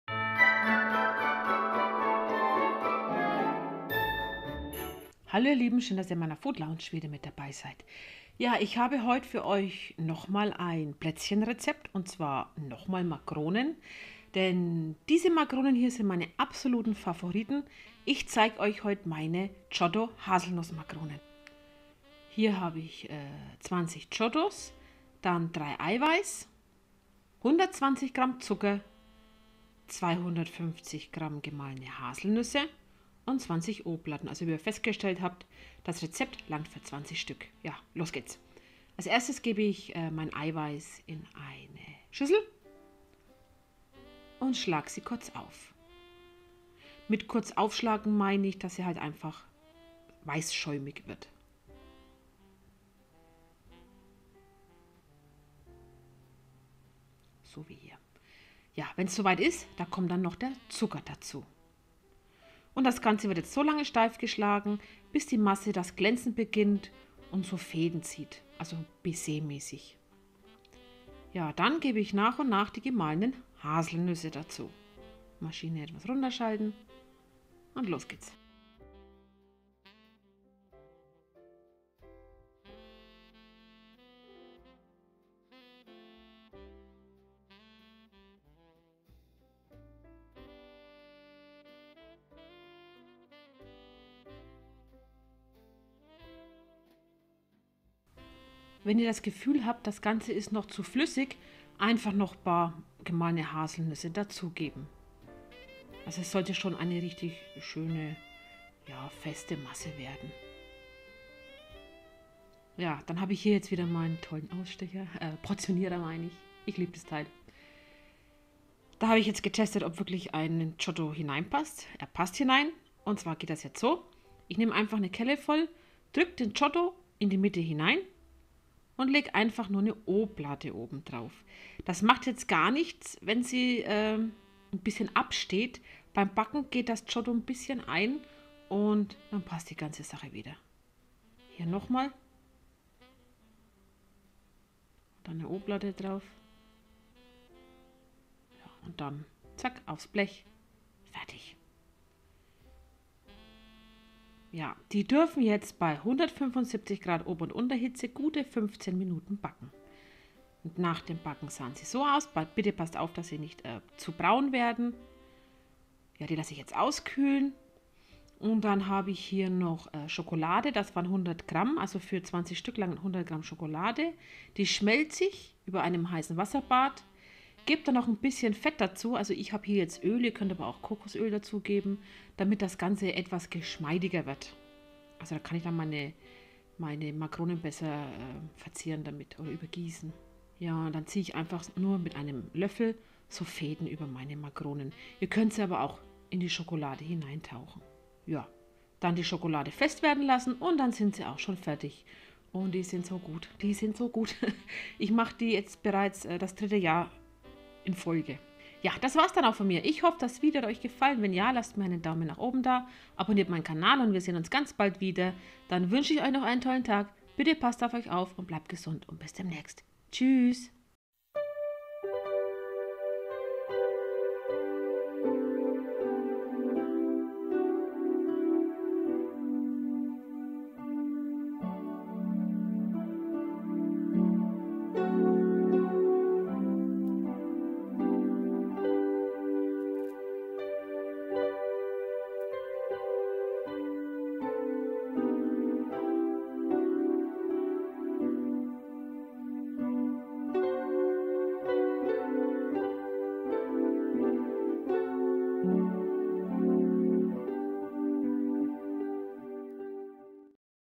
Giotto Haselnussmakronen/ Weihnachtsbäckerei/ ganz wenig Zutaten!
Hallo Zusammen!
Heute gibt es eins meiner persönlichen Favoriten Rezepte und zwar zeige ich euch  meine Giotto Haselnussmakronen.
Viel Spaß
P.S.
Wer keinen Makronen-Portionierer hat, füllt die Masse einfach in einen Spritzbeutel, legt das Giotto auf die Oblate und spritz die Masse darauf.
Für 20 Stück braucht man:

20 Giottos ( Unbezahlte Werbung)
3 Eiweiß
120 gr. Zucker
250 gr. gem. Haselnüsse
20 Oblaten ( 4cm)
100 gr. Schokolade ( Geschmack nach Wahl!)
Ofen auf 175 Grad O/U Hitze vorheizen/ 10-15 Minuten backen.